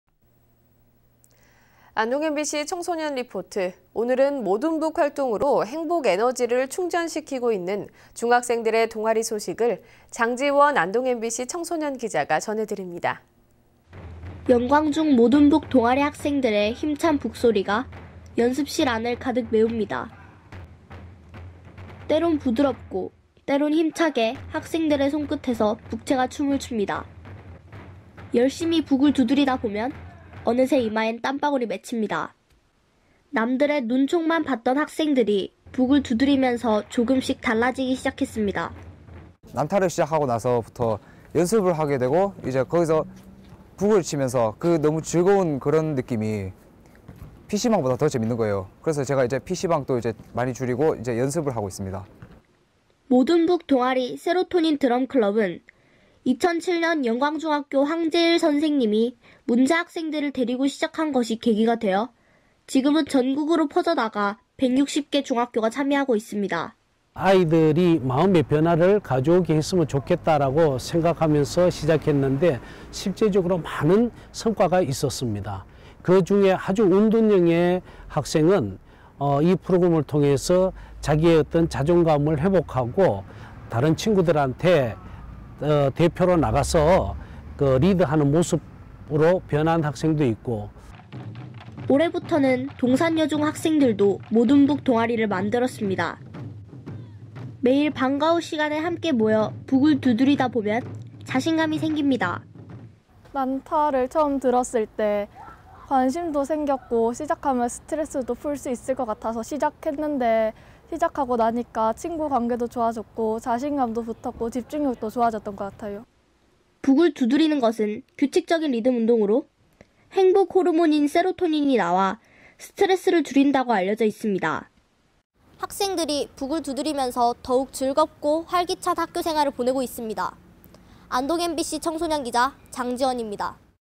[안동MBC뉴스]모듬북활동으로 행복에너지 충전
2015/11/20 11:14:14    작성자 : 보도팀장

◀ＡＮＣ▶

안동MBC청소년리포트.

오늘은 모듬북활동으로 행복에너지를
충전시키고 있는 중학생들의 동아리소식을
장지원 안동MBC청소년기자가 전해드립니다.
◀ＥＮＤ▶

영광중 모듬북 동아리 학생들의
힘찬 북소리가 연습실 안을 가득 메웁니다.

때론 부드럽고 때론 힘차게
학생들의 손끝에서 북채가 춤을 춥니다.

열심히 북을 두드리다보면
어느새 이마엔 땀방울이 맺힙니다.

남들의 눈총만 받던 학생들이
북을 두드리면서 조금씩 달라지기 시작했습니다.

◀ＩＮＴ▶ 최원혁 / 영광중학교 3학년
“난타를 시작하고 나서부터 연습을 하게 되고 북을 치는 것이 느낌이 피씨방 보다 더 재미있었어요. 그래서 피씨방에 가는 것을 많이 줄이고 연습을 하는 데에 많은 시간을 쓰고 있습니다.”

세로토닌드럼클럽은
2007년 영광중학교 황재일 선생님이
문제 학생들을 데리고 시작한 것이 계기가 돼
지금은 전국으로 퍼져나가
160개 중학교가 참여하고 있습니다.

◀ＩＮＴ▶ 황재일 / 영광중학교 교사
“아이들의 마음을 변화할 수 있었으면 좋겠다고 생각하면서 시작했는데 실질적으로 많은 성과가 있었습니다. 그 중에 은둔형이었던
한 학생은 이 프로그램을 통해서 자신의
자존감을 회복하고 친구들 앞에 대표로 나가서 리드하는 모습으로 변한 학생도 있고...”

올해부터는 동산여중 학생들도
모듬북 동아리를 만들었습니다.

매일 방과 후 시간에 함께 모여 북을 두드리다보면 자신감이 생깁니다.

◀ＩＮＴ▶ 배현희 / 동산여자중학교 2학년
“난타를 처음 들었을 때 관심도 생겼고 시작하면 스트레스도 풀 수 있을 것 같아서 시작했는데 시작하고 나니까 친구관계도 좋아졌고 자심감도 붙었고 집중력도 생긴 것 같아요.”

북을 두드리는 것은 규칙적인 리듬 운동으로
행복 호르몬인 세로토닌이 나와 스트레스를 줄인다고 알려져 있습니다.

학생들이 북을 두드리면서
더욱 즐겁고 활기찬 학교생활을 하고 있습니다.
안동MBC 청소년기자 장지원입니다.